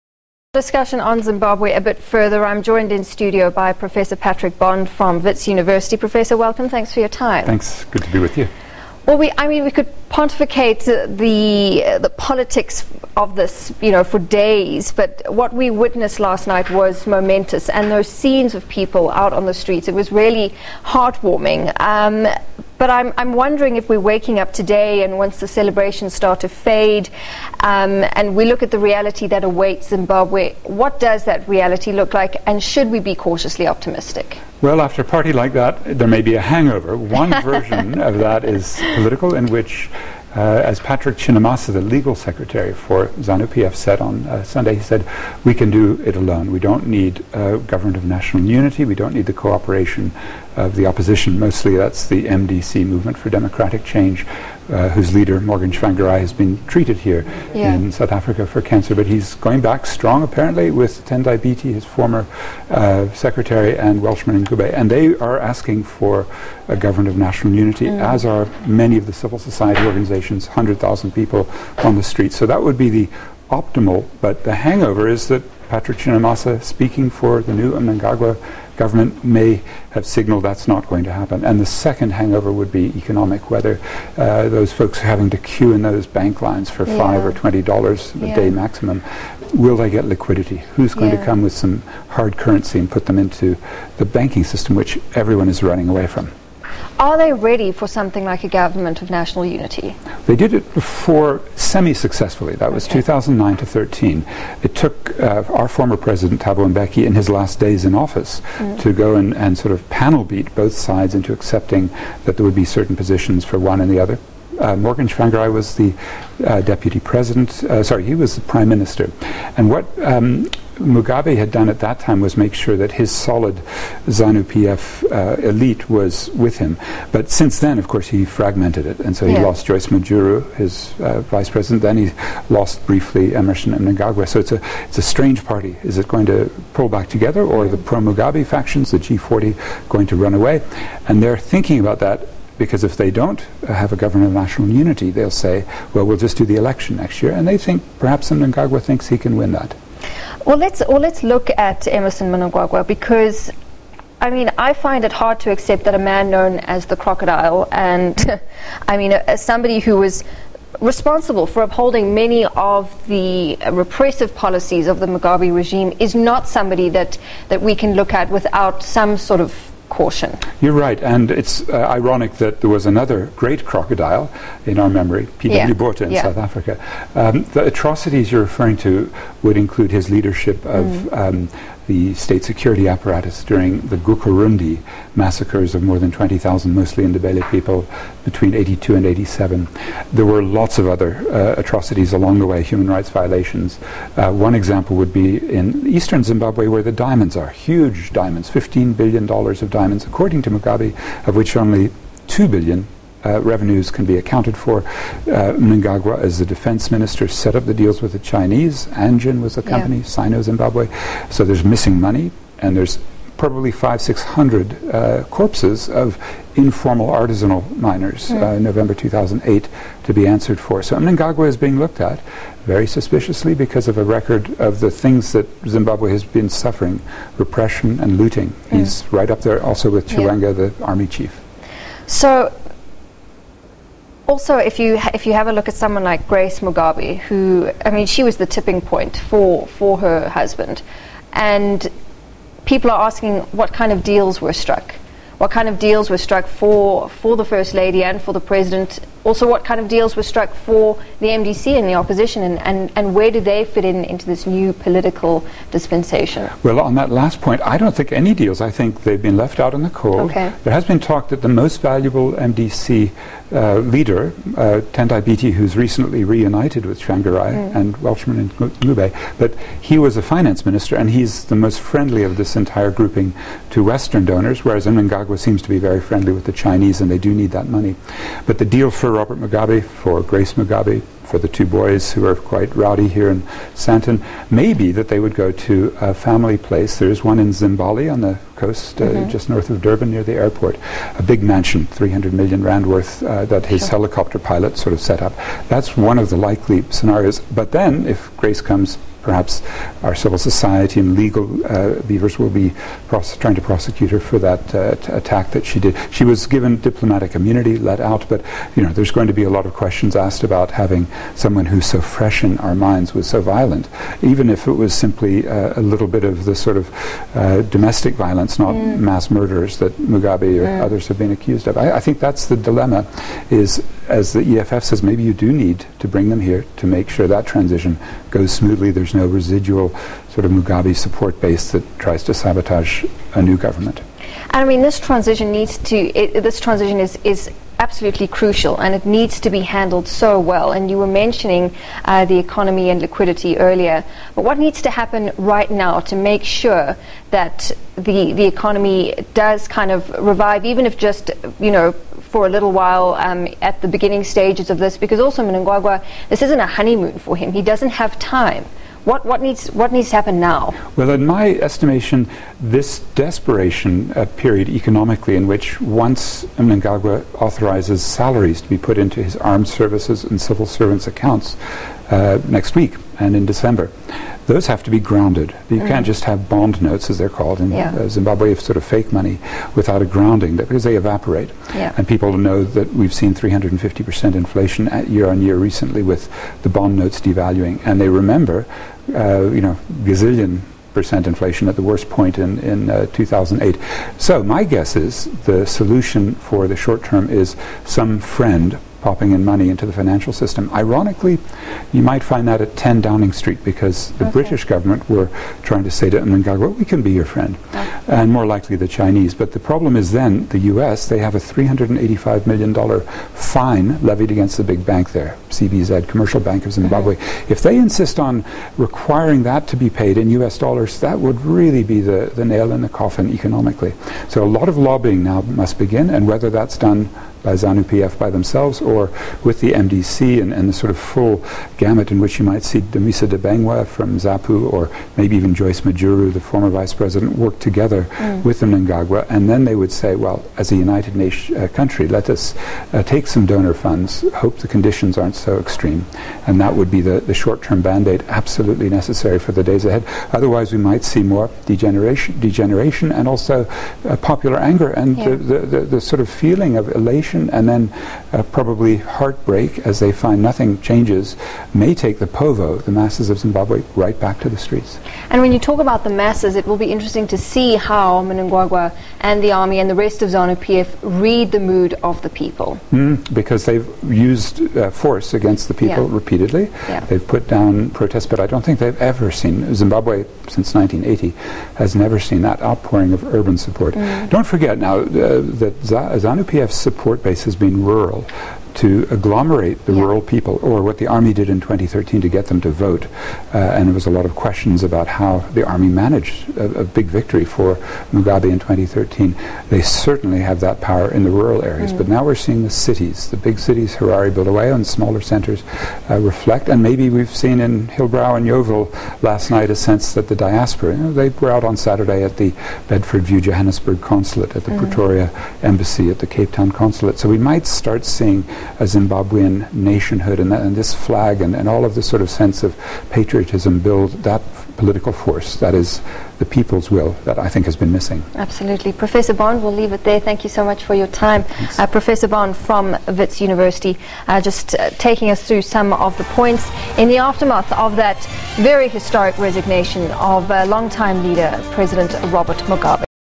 Professor Patrick Bond on Zimbabwe
To take our discussion on Zimbabwe a bit further, we are joined in studio by Professor Patrick Bond from Wits University.